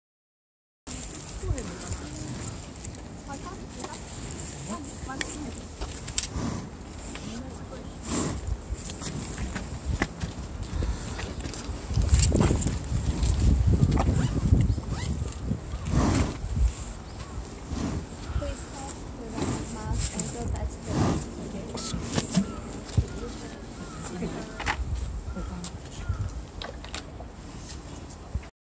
アドベンチャーワールドのシロサイ
アドベンチャーワールドのシロサイは賢くて、餌を見せると口を開けてくれました。